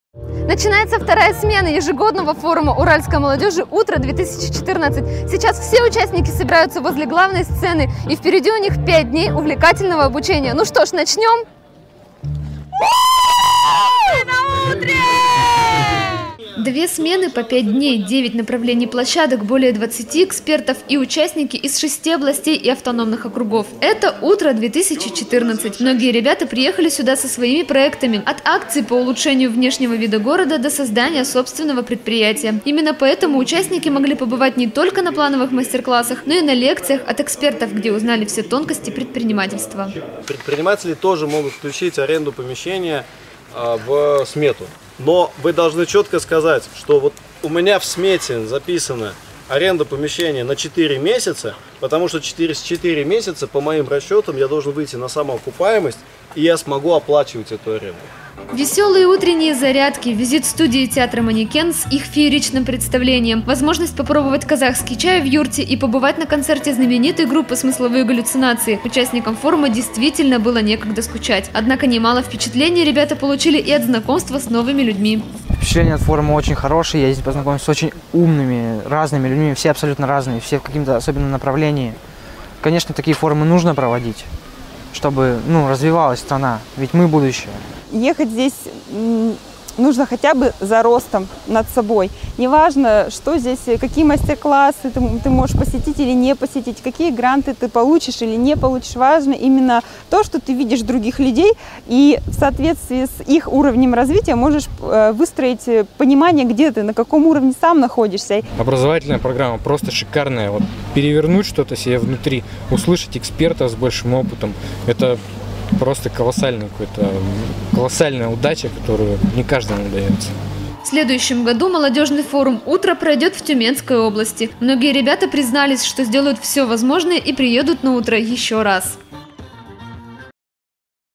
Форум уральской молодежи УТРО-2014
2 смены по 5 дней, девять направлений площадок, более 20 экспертов и участники из шести областей и автономных округов. Это -- УТРО-2014. Многие ребята приехали сюда со своими проектами -- от акций по улучшению внешнего вида города до создания собственного предприятия. Именно поэтому участники могли побывать не только на плановых мастер-классах, но и на лекциях от экспертов, где узнали все тонкости предпринимательства.
Веселые утренние зарядки, визит студии-театра МАНЕКЕН с их фееричным представлением, возможность попробовать казахский чай в юрте и побывать на концерте знаменитой группы СМЫСЛОВЫЕ ГАЛЛЮЦИНАЦИИ -- участникам форума действительно было некогда скучать. Однако немало впечатлений ребята получили и от знакомства с новыми людьми.
В следующем году молодежный форум УТРО пройдет в Тюменской области. Многие ребята признались, что сделают все возможное и приедут на УТРО еще раз.